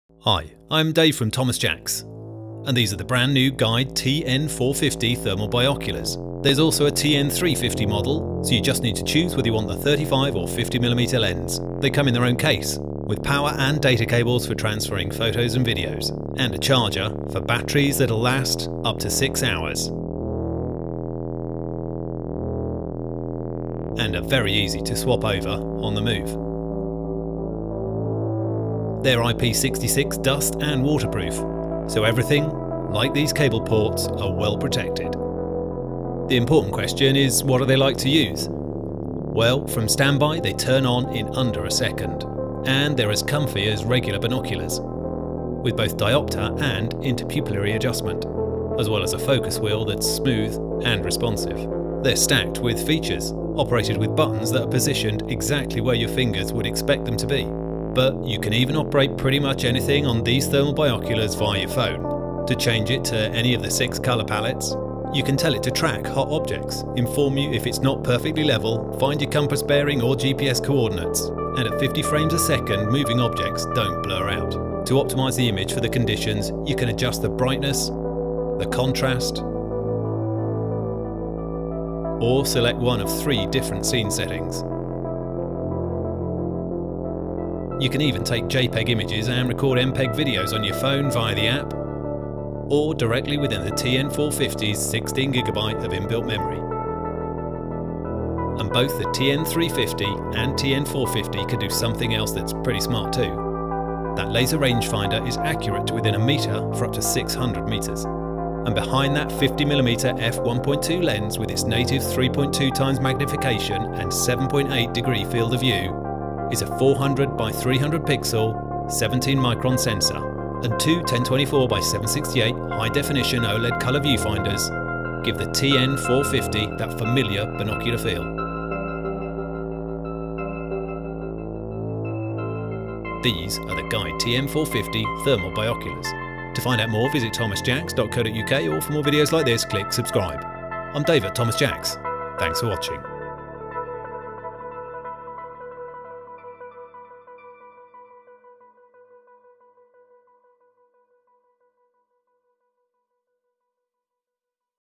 New for 2022. Guide TN450 thermal binoculars
Due for their UK release early in 2022, here's the first look at the brand new TN450 thermal bioculars from Guide.

They really show that two eyes are better than one when it comes to usability; especially when they're this packed with features, that include a deadly accurate laser range finder.